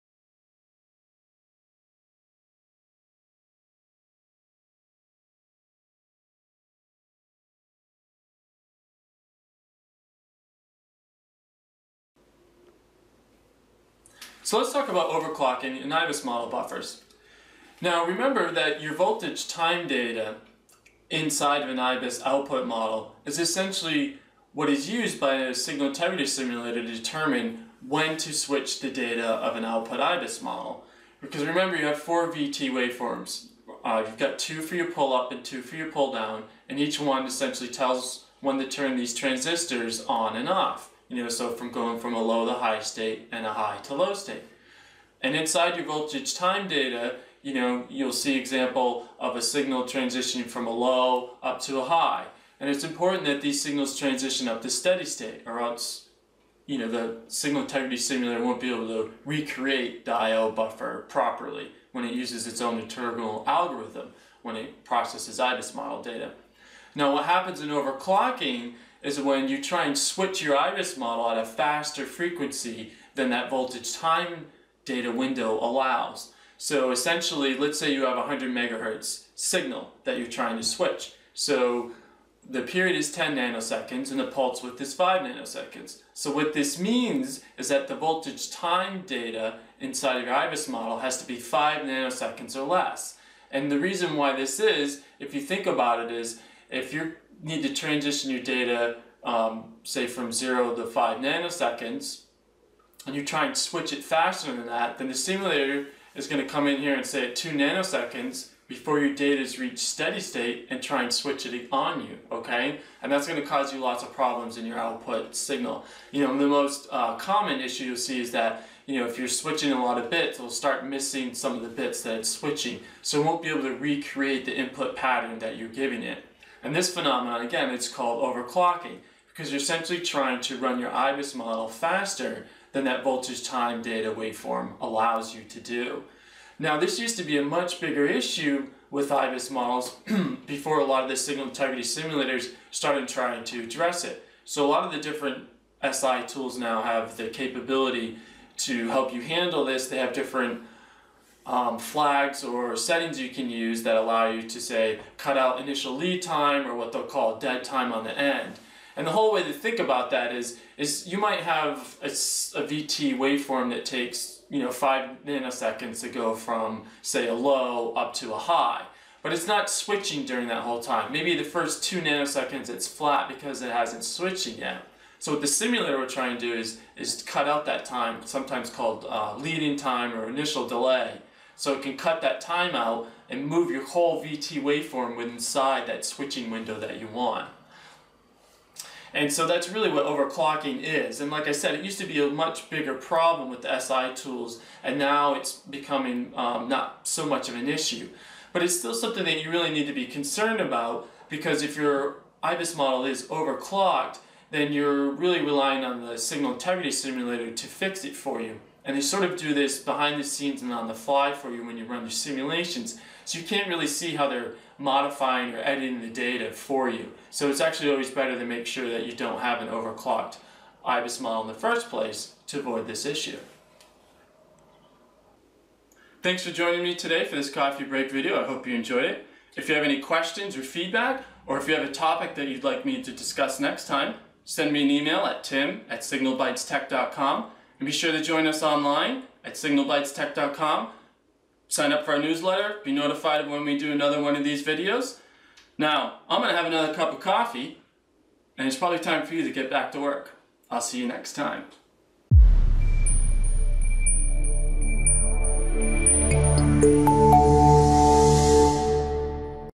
Coffee Quick Tip Overclocking IBIS Model
Understand how overclocking and IBIS model works and why it's an issue.